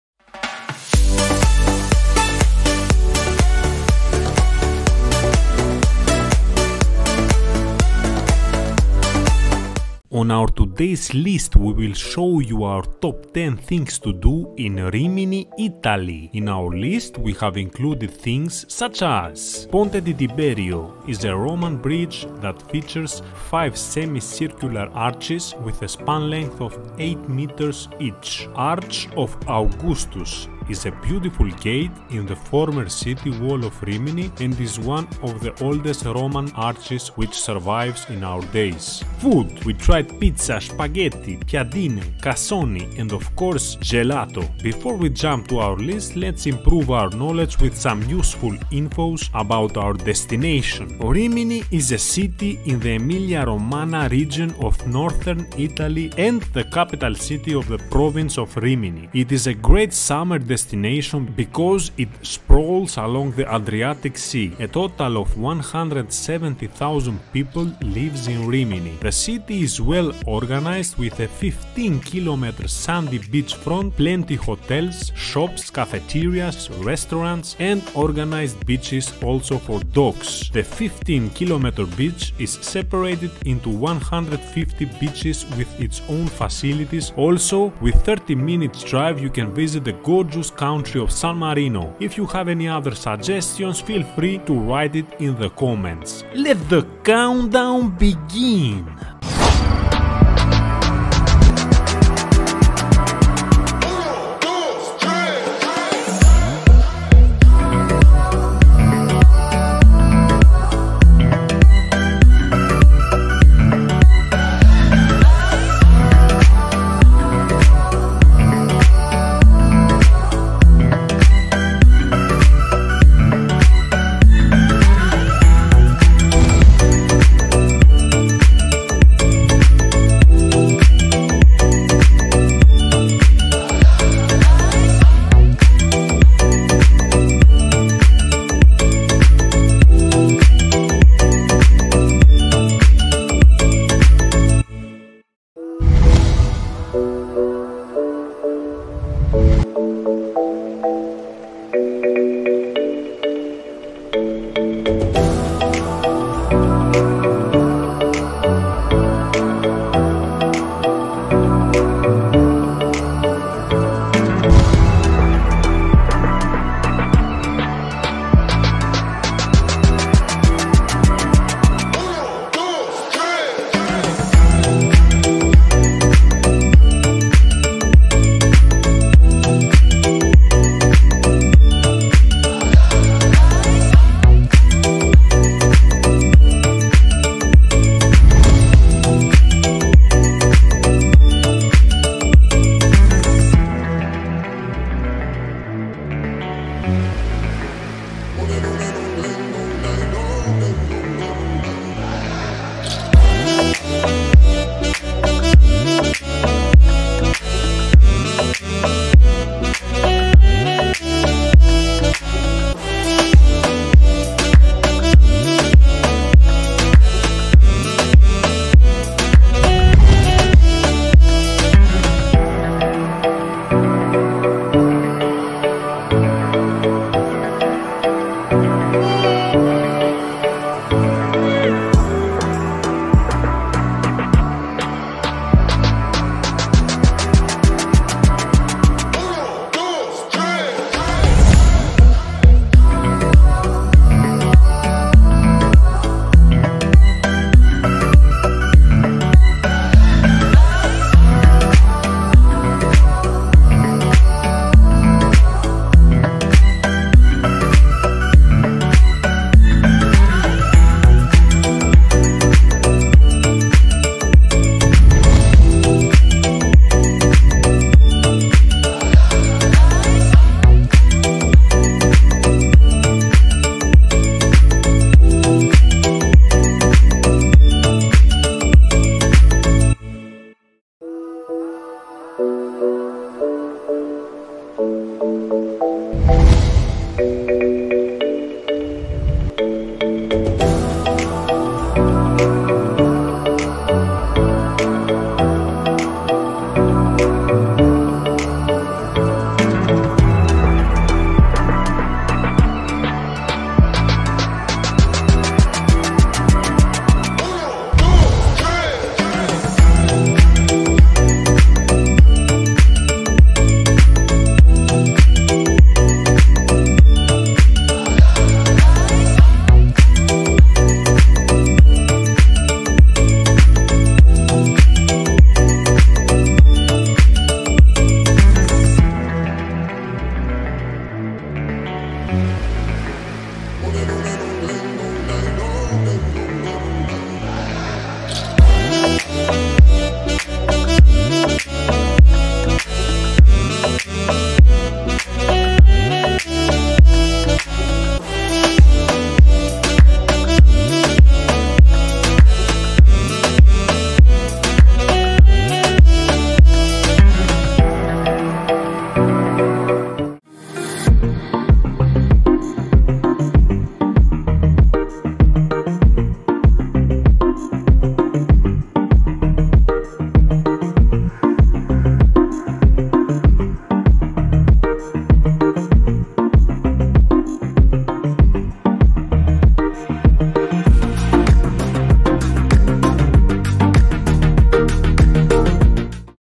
Top 10 Things To Do in Rimini Italy
This list is dedicated to Tripadvisor,Google Guides and Tourists MUST DO guide lists but also a private list of Travel Time Youtube channel for Rimini Italy

T-shirt with yor own Logo
Selfie Stick
Map Scratch
Bluetooth Headphones
Passport Holder

Timestamps:
0:00 Intro
1:39 Castel Sismondo
2:07 Italia in Miniatura
2:26 Fiabilandia
2:45 Mirabilandia
3:08 Rimini Beaches
3:40 La Ruota
4:01 Arch of Augustus
4:25 San Marino Daytrip
4:47 Food
5:09 Ponte di Tiberio
6:01 Outro